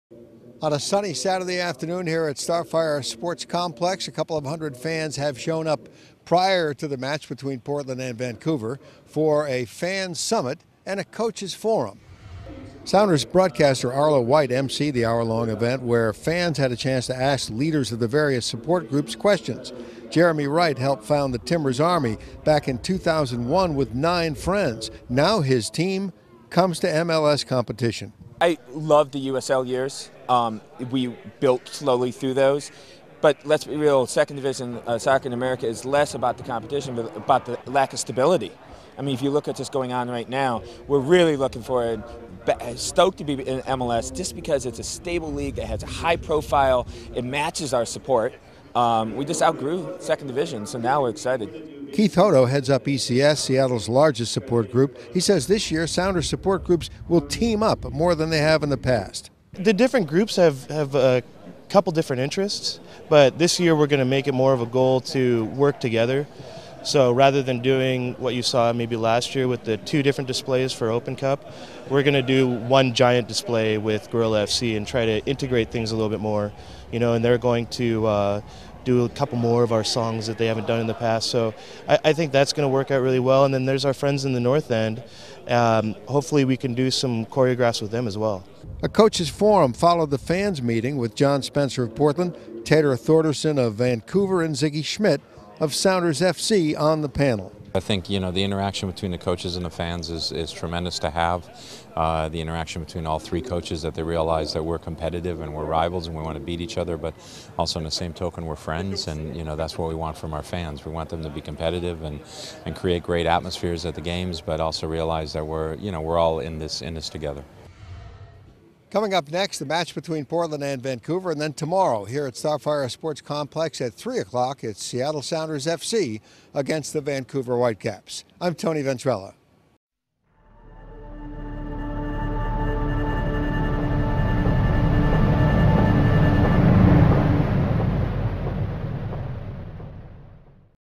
Cascadia Supporters Summit
The inaugural Cascadia Supporters Summit was held at Starfire Sports Complex as host Arlo White discussed the renewed rivalry with fans from all three cities.